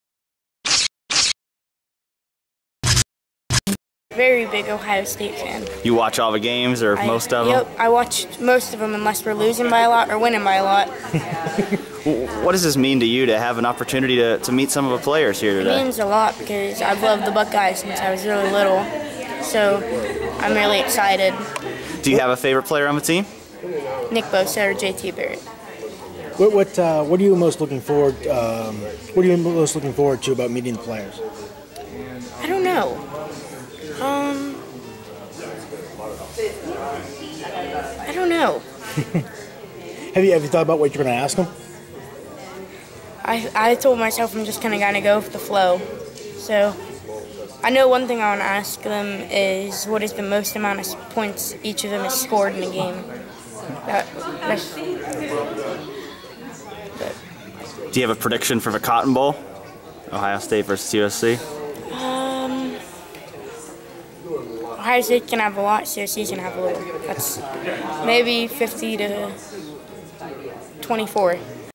Rayne McCann meets Ohio State football team at Nationwide Children's Hospital - ELEVENWARRIORS.COM
Rayne McCann was among the Nationwide Children's Hospital patients who had the opportunity to meet Urban Meyer and members of the Ohio State football team on Monday.